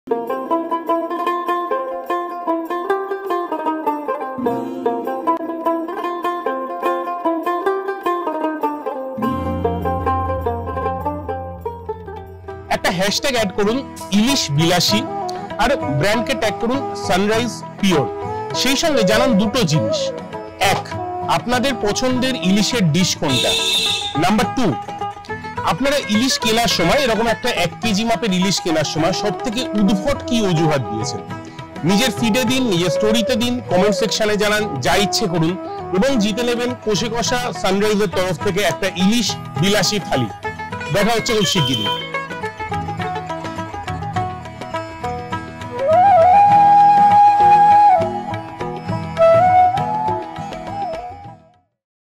Sunrise Pure IlishBilashi festival is on!
Tell us what is your favourite ilish dish and what is the funniest excuse you have given for bargaining one Hilsa fish from the market.

Share your story on your social media, but keep 2 things in mind.
Use ilibilashi and tag @sunrisepure

And get a chance to win ilish thali for 2 from Koshe Kosha restaurant.

Remember, good Hilsa needs SunrisePure